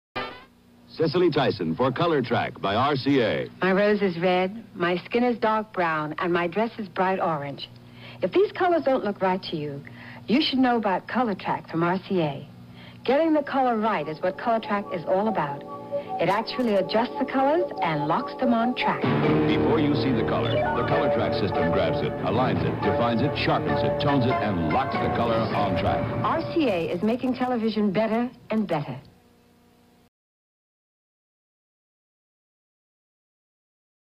RCA ColorTrak TV Commercial (Cicely Tyson, 1977)
"RCA is making television better and better."
RCA ColorTrak television set commercial featuring actor Cicely Tyson ('Sounder,' 'Roots'). Spot aired in October 1977.